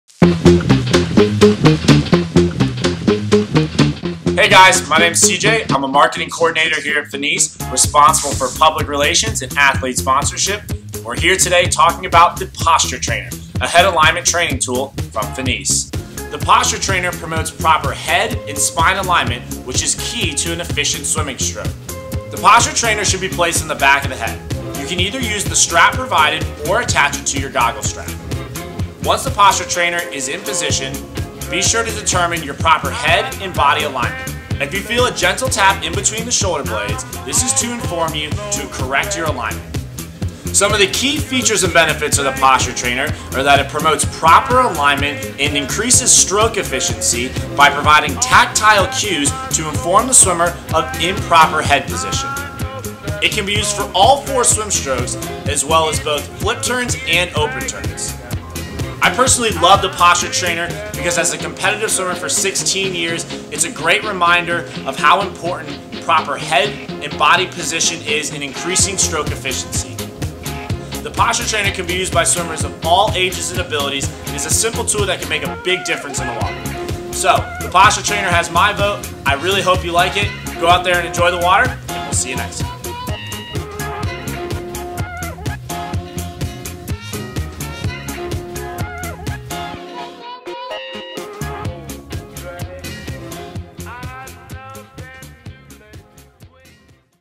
FINIS Posture Trainer
Posture Trainer
Head Alignment Training Tool

Promotes correct head and spine alignment. When secured to the back center of the head, the device should be undetectable until the swimmer exceeds the recommended posture range. Improves alignment for all four swim strokes.